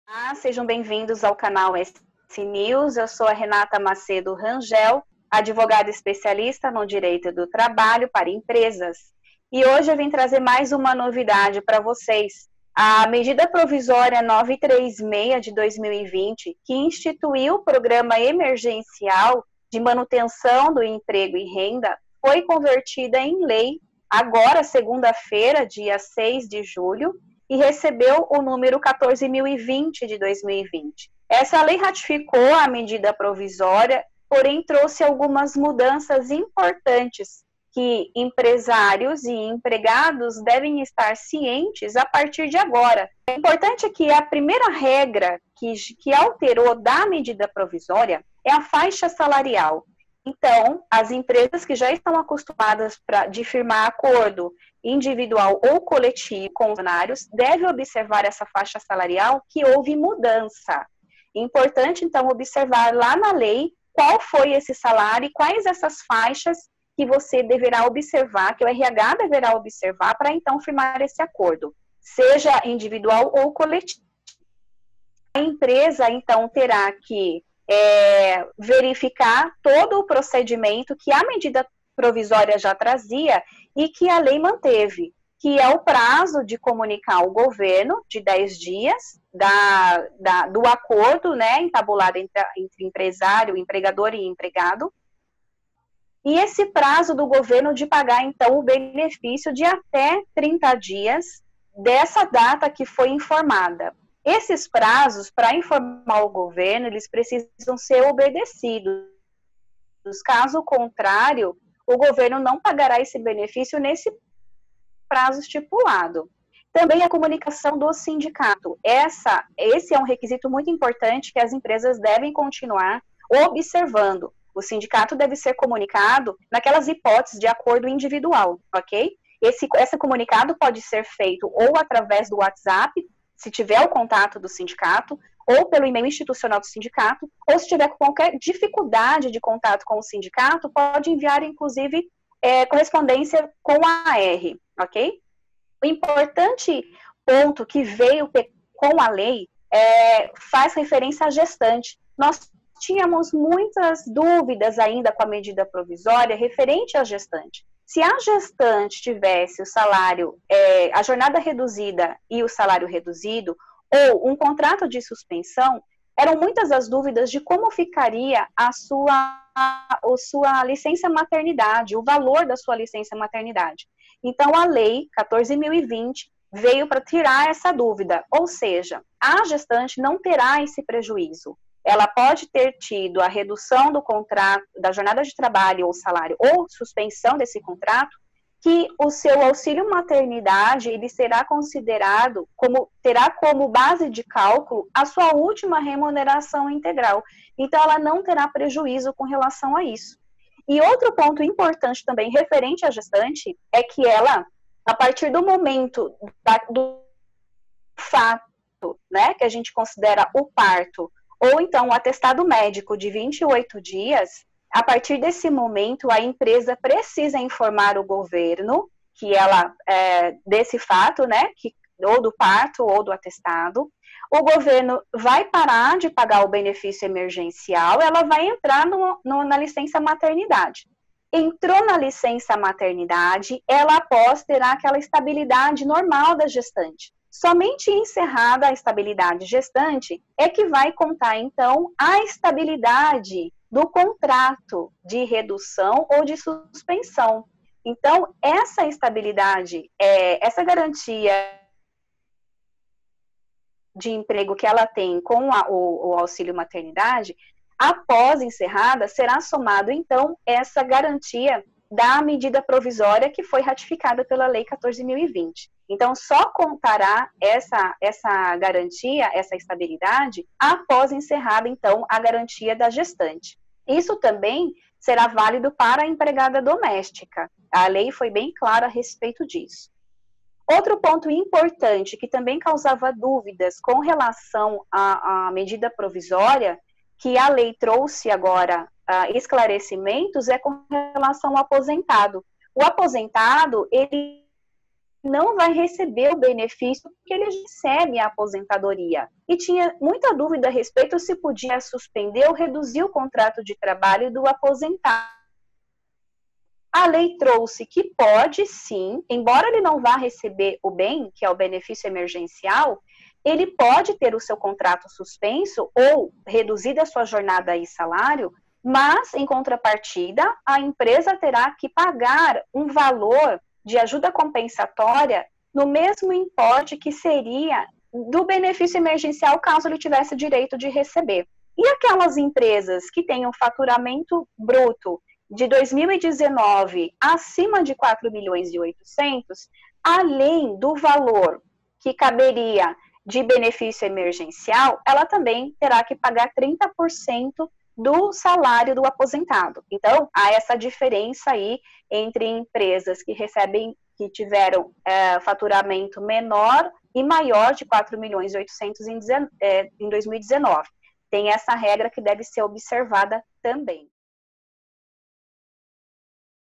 Lei 14.020/2020 traz alterações referentes à gestante e ao aposentado
Advogada Renata Macedo Rangel, especialista em Direito Trabalhista para as Empresas, explica os desdobramentos da MP nº 936/2020 que instituiu o Programa Emergencial de Manutenção do Emprego e Renda do governo foi convertida em Lei.
O prolongamento do estado de Calamidade Pública no Brasil devido a pandemia do coronavírus exige ajustes na Lei, direitos e deveres das empresas e trabalhadores. Neste vídeo a advogada aprofunda o que mudou sobre detalhes como suspensão de contrato de trabalho, redução de salários envolvendo desde o trabalhador, a gestante e o aposentado.